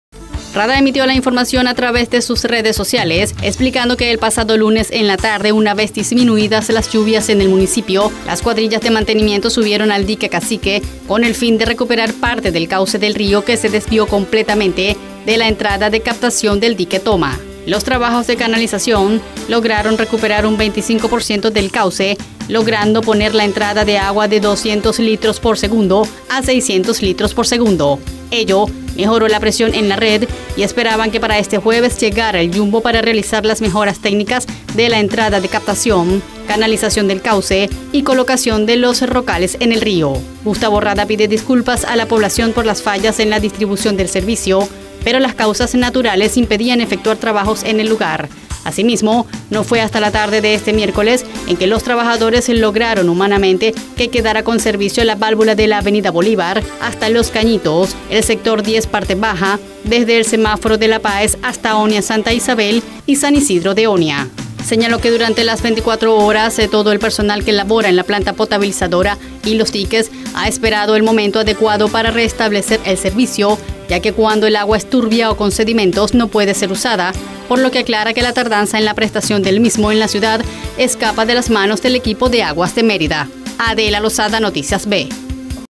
RESTITUYEN DE MANERA PROGRESIVA SERVICIO DE AGUA EN EL VIGÍA
EL GERENTE GENERAL DE AGUAS DE MÉRIDA, GUSTAVO RADA, INFORMÓ SOBRE LOS TRABAJOS QUE SE REALIZAN EN LOS DIQUES QUE SURTEN DE AGUA A LA PLANTA POTABILIZADORA DE MUCUJEPE Y EXPLICÓ EL PORQUÉ DEL RETRASO EN EL SUMINISTRO DE AGUA A LA CIUDAD